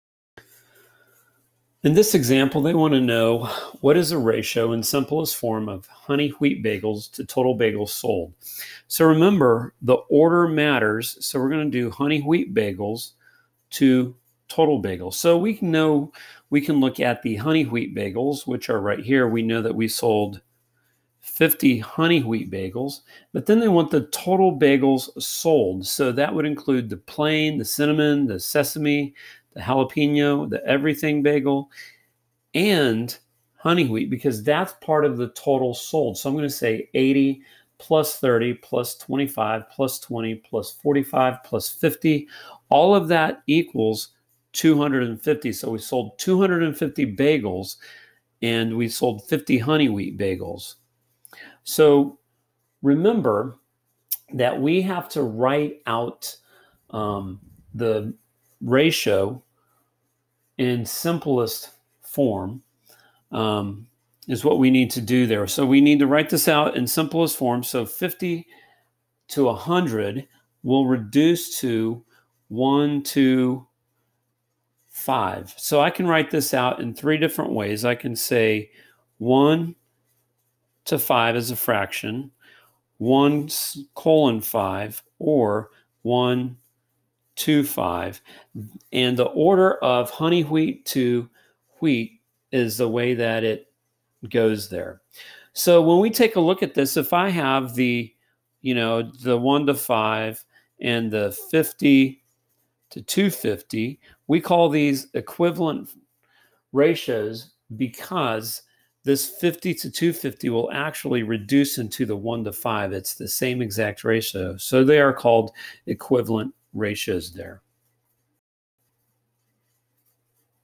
GM CW 08.1 Example 2
Writing ratios in simplest form and in three different ways